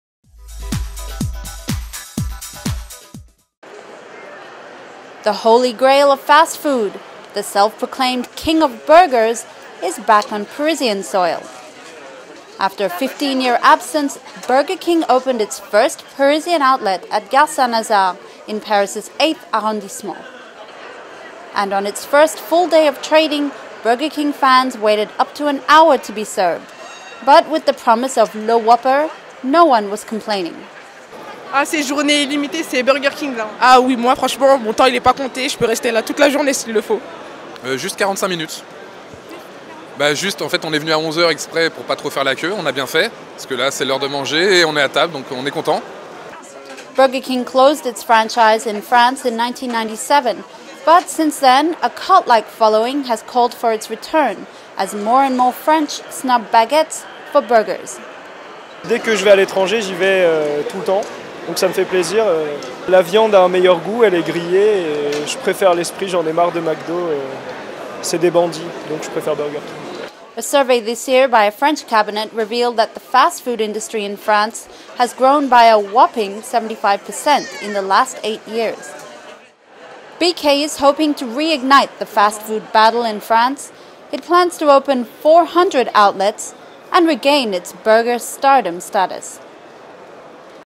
Burger King's triumphant return to Paris
Burger King made a much-awaited return to Paris this week, with the first outlet opening at Gare St-Lazare. And people (happily) queued up for an hour to get a taste of Le Whopper! By Parisian News TV